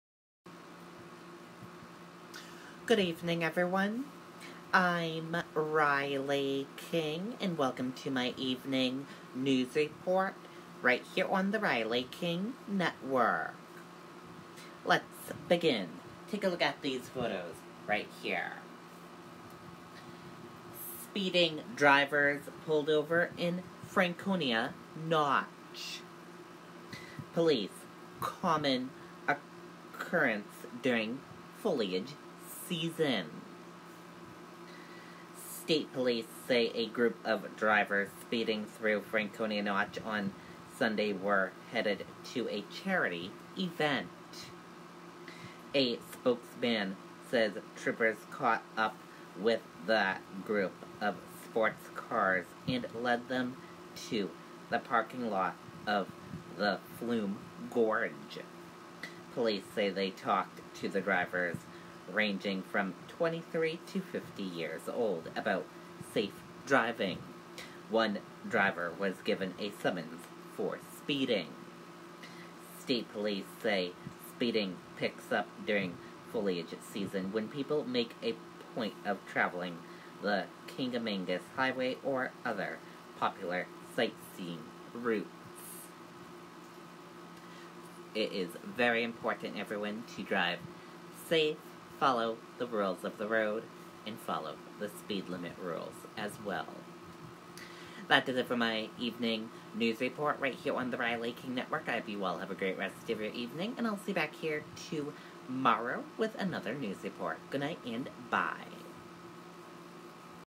Speeding drivers pulled over in Franconia Notch
9/22/19